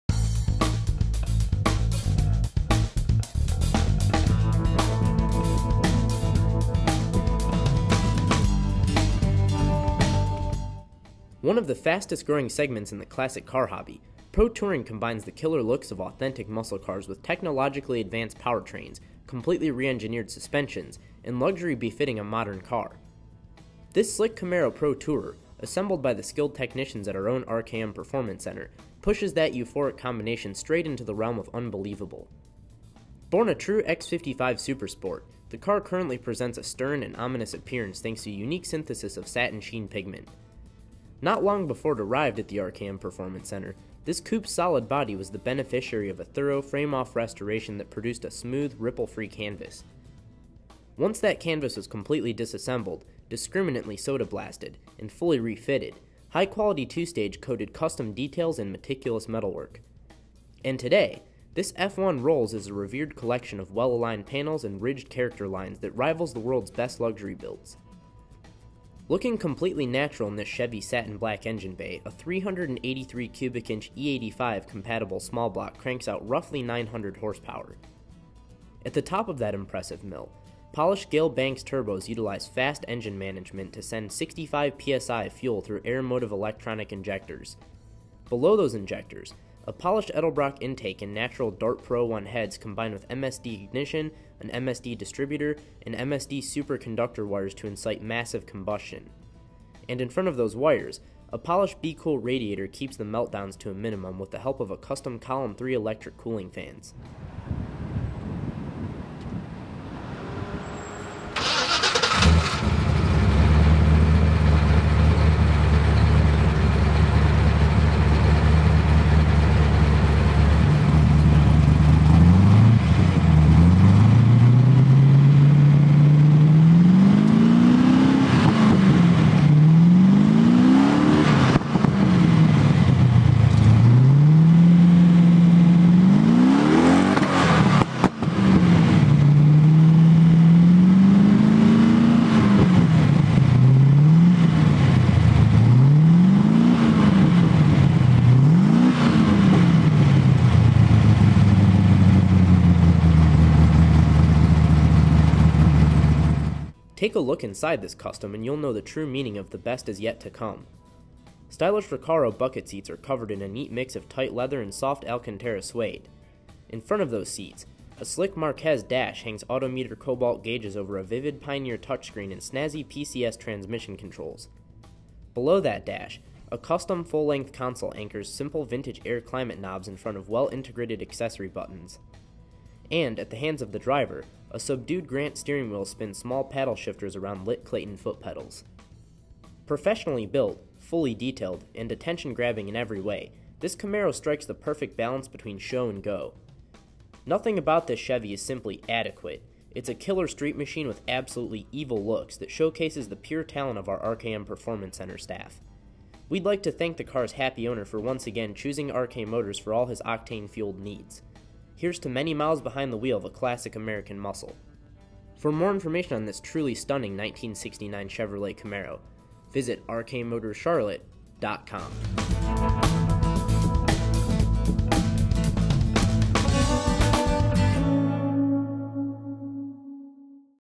S00141 / 1969 Chevrolet Camaro
One of the fastest-growing segments in the classic car hobby, pro-touring combines the killer looks of authentic muscle cars with technologically advanced powertrains, completely re-engineered suspensions and luxury befitting a modern car. This slick Camaro pro-tourer, assembled by the skilled technicians at our own RKM Performance Center, pushes that euphoric combination straight into the realm of unbelievable!

Born a true X55 Super Sport, the car currently presents a stern and ominous appearance thanks to a unique synthesis of satin sheen pigment. Not long before it arrived at the RKM Performance Center, this coupe's solid body was the beneficiary of a thorough, frame-off restoration that produced a smooth, ripple-free canvas. Once that canvas was completely disassembled, discriminately soda blasted and fully re-fitted, high quality two-stage coated custom details and meticulous metalwork. And today, this F1 rolls as a revered collection of well-aligned panels and ridged character lines that rivals the world's best luxury builds.

Looking completely natural in this Chevy's Satin Black engine bay, a 383 cubic inch, E85 compatible small block cranks out roughly 900 horsepower. At the top of that impressive mill, polished Gale Banks turbos utilize F.A.S.T. engine management to send 65 psi of fuel through Aeromotive electronic injectors. Below those injectors, a polished Edelbrock intake and natural Dart Pro 1 heads combine with MSD ignition, an MSD distributor and MSD Super Conductor wires to incite massive combustion. And in front of those wires, a polished Be Cool radiator keeps the meltdowns to a minimum with the help of a custom cowl and three electric cooling fans.

Behind the brazen V8, a Bowler-built 4L80E 4-speed twists power to a 9-inch Ford rear end. That narrowed pumpkin spins between Detroit Speed tubs, Detroit Speed springs and high quality AFCO shocks. Turns come courtesy of well-mannered rack and pinion steering. Stops are provided by a new Baer system that's augmented with 15-inch, drilled and slotted rotors. And the finishing touch is a sweet quartet of custom Asanti 5-spokes that spin 245/35R19 Pirelli P Zeros in front of 335/30ZR20 Pirelli P Zeros.

Take a look inside this stunning custom and you'll know the true meaning of "the best is yet to come". Stylish Recaro buckets are covered in a neat mix of tight leather and soft Alcantara suede. In front of those seats, a slick Marquez dash hangs Auto Meter Cobalt gauges over a vivid Pioneer touchscreen and snazzy PCS transmission controls. Below that dash, a custom, full-length console anchors simple Vintage Air climate knobs in front of well-integrated accessory buttons. And, at the hands of the driver, a subdued Grant steering wheel spins small paddle shifters around lit Clayton foot pedals.

Professionally built, fully detailed and attention-grabbing in every way, this Camaro strikes the perfect balance between show and go. Nothing about this Chevy is simply 'adequate'; it's a killer street machine with absolutely evil looks that showcases the pure talent of our RKM Performance Center staff. We'd like to thank the car's happy owner for once again choosing RK Motors for all his octane-fueled needs. Here's to many miles behind the wheel of classic American muscle!